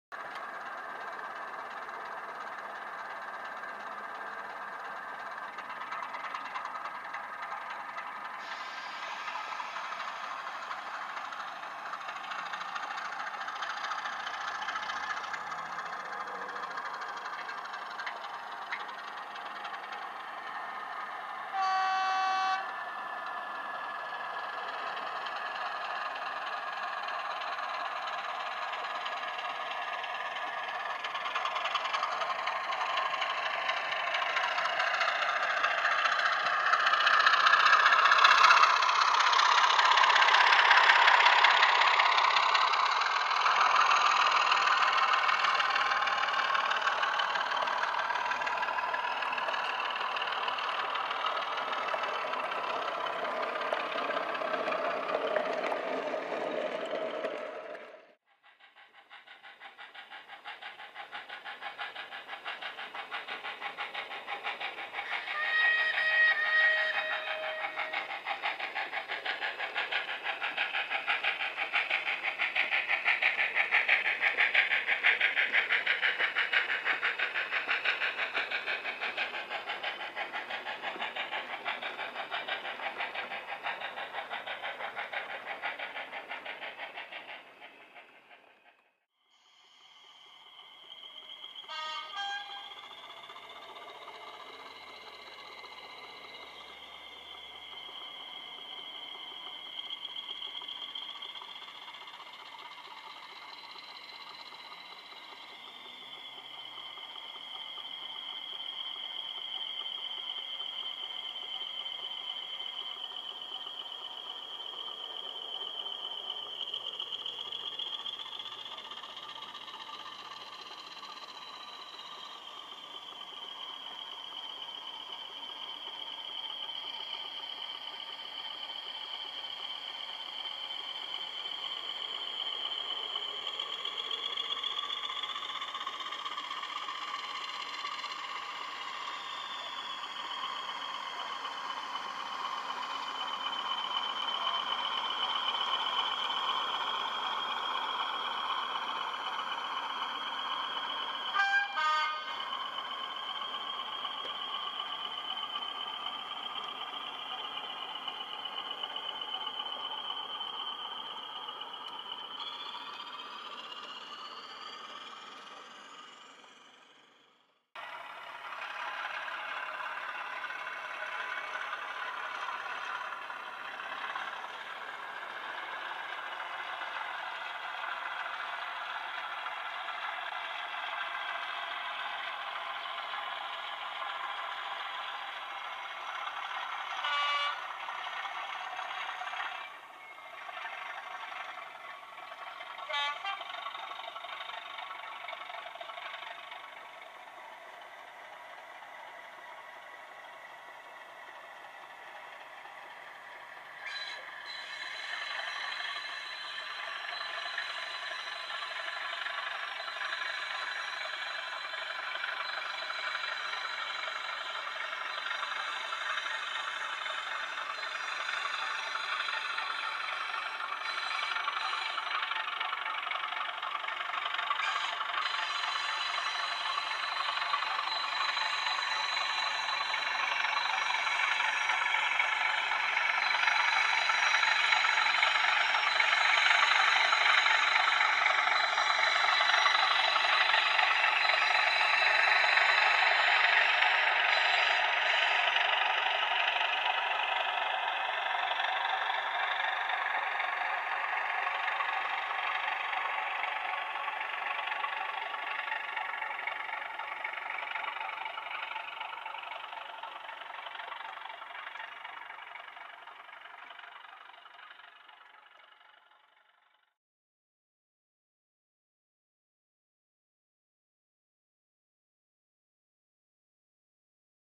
EM gauge model railway part2
More views of my now defunct EM gauge layout.